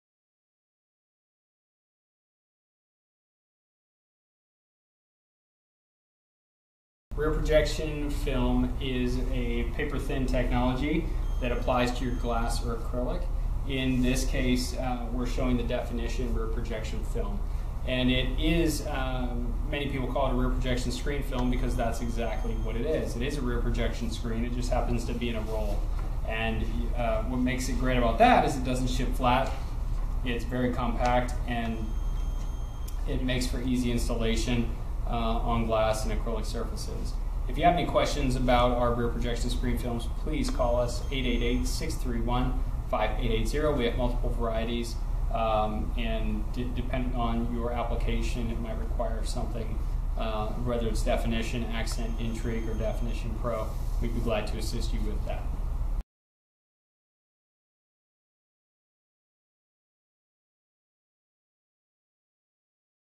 Rear Projection Film Screens
Rear Projection Screen Films take the rigid rear projection screen products and put it into a film version that can be applied to any glass or acrylic surface. Rigid rear projection screens we the staple of store front window displays for years but did cost more to pack and ship because they had to be crated and ship via freight for most standard sizes, Rear Projection Screen films are able to be rolled and shipped in tubes or boxes and even full rolls of 60”x33’ can be shipped with standard shipping companies for cost effective delivery. Using a Rear Projection Screen film on glass or acrylic gives you the same effect as a rigid rear projection screen would but with less shipping and crating costs.